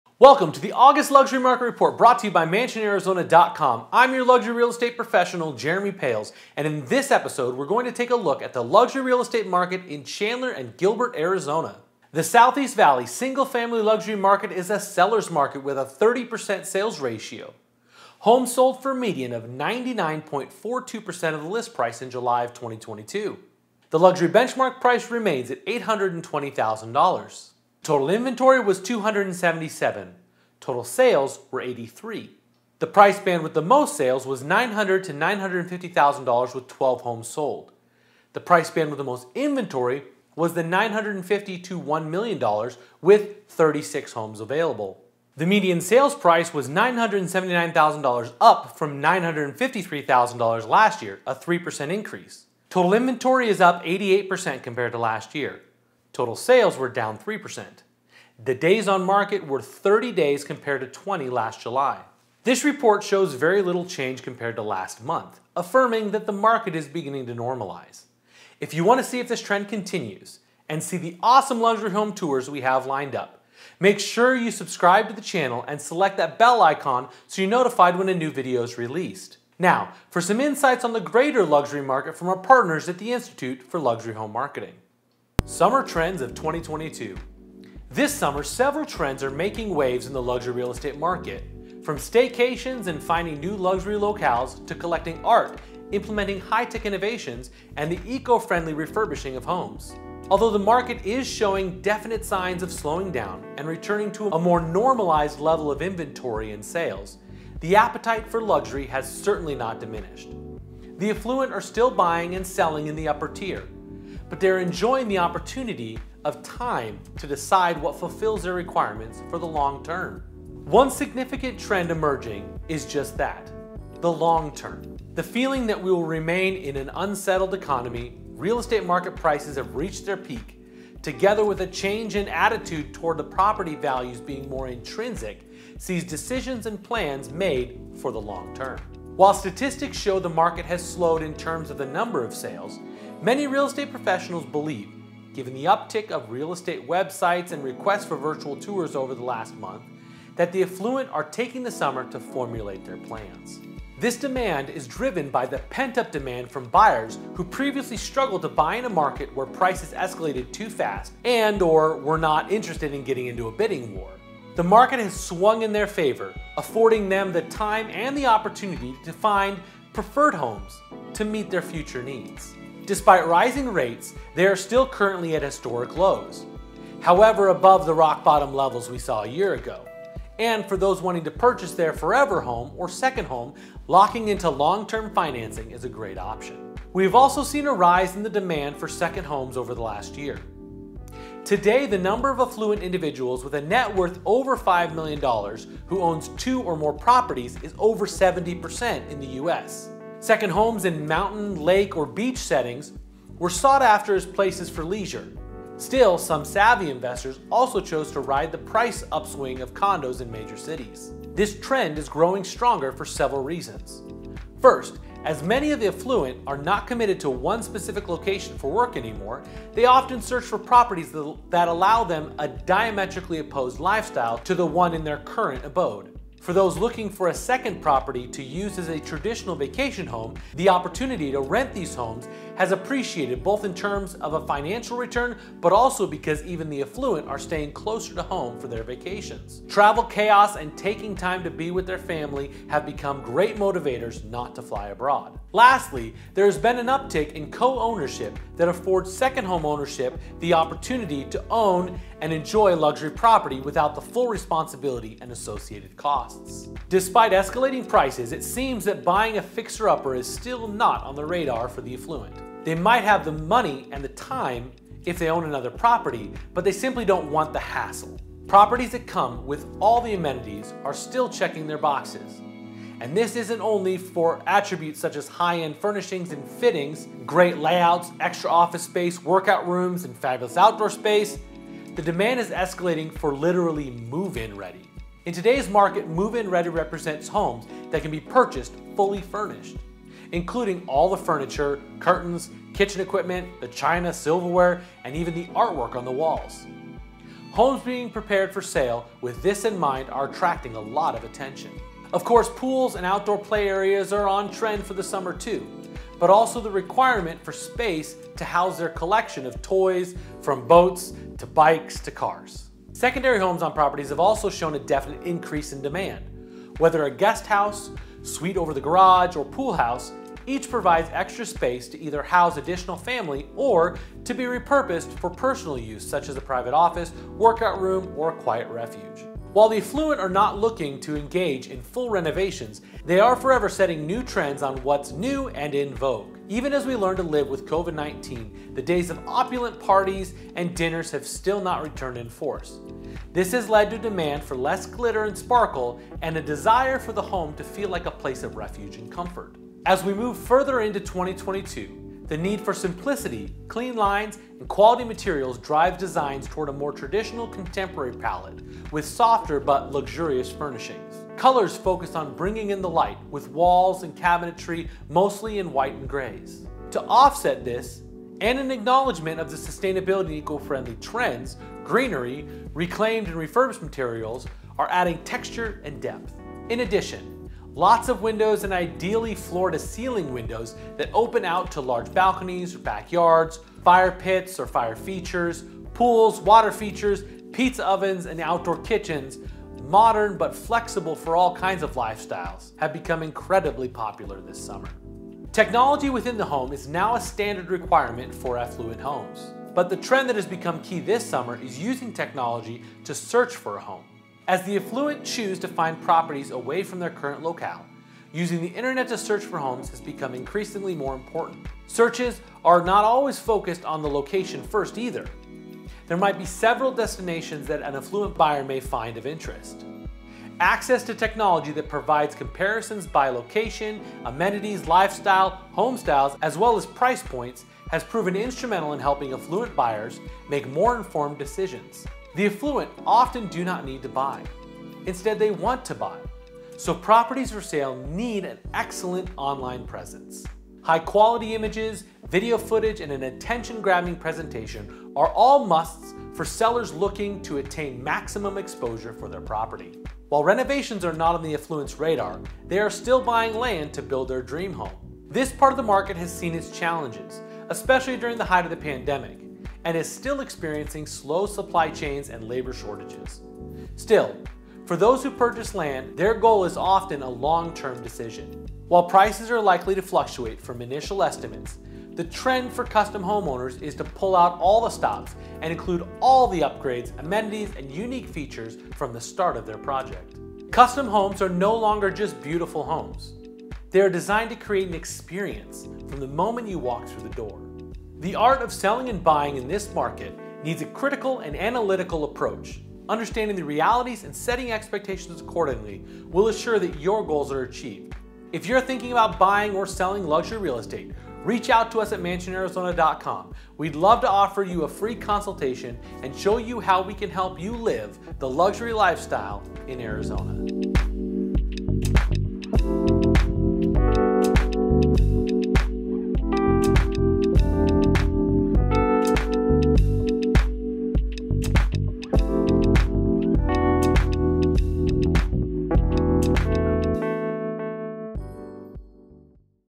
Southeast Valley Luxury Market Report | August 2022
Welcome to the August Luxury Market report brought to you by Mansionarizona.com. I’m your Luxury Real Estate Professional, Jeremy Poehls, and in this episode, we are going to take a look at the Luxury Real Estate Market in Chandler and Gilbert, Arizona.

Southeast Valley
-The Southeast Valley single-family luxury market is a Seller's Market with a 30% Sales Ratio.
-Homes sold for a median of 99.42% of the list price in July 2022.
-The Luxury Benchmark Price remains at $820,000 Dollars
-Total Inventory was 277
-Total Sales were 83
-The price band with the most sales was 900-950,000 dollars with 12 homes sold
-The price band with the most inventory was 950-1 Million dollars with 36 homes available.
-Median Sales price was $979,000 up from $953,000 last year, a 3% increase
-Total inventory is up 88% compared to last year
-Total sales were down 3%
-Days on Market were 30 days compared to 20 last July

This report shows very little change compared to last month...affirming that the market is beginning to normalize.  If you want to see if this trend continues and see the awesome Luxury Home Tours we have lined up, make sure you subscribe to the channel and select the bell icon, so you're notified when a new video is released.

Now for some insights on the greater luxury market from our partners at the Institute for Luxury Home Marketing.

Jeremy Poehls - Realtor®
Certified Luxury Home Marketing Specialist
MansionArizona.com brokered by eXp Realty